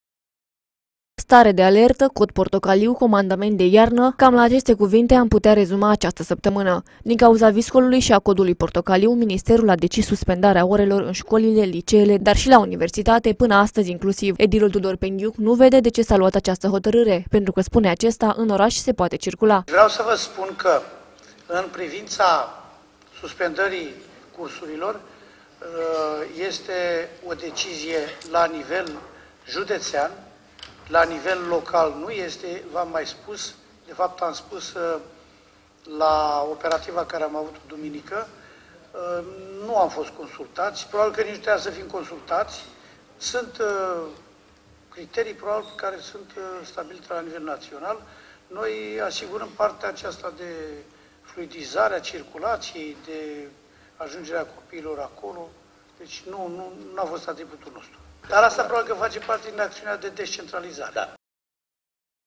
Judetul Arges s-a aflat sub cod portocaliu in aceasta saptamana
Judetul Arges s-a aflat sub cod portocaliu in aceasta saptamana , motiv pentru care autoritatile au decis inchiderea scolilior din judet. Cu toate acestea , edilul spune ca orele nu trebuiau suspendate intrebandu-se retoric daca nu cumva acest proces face parte din cel al descentralizarii.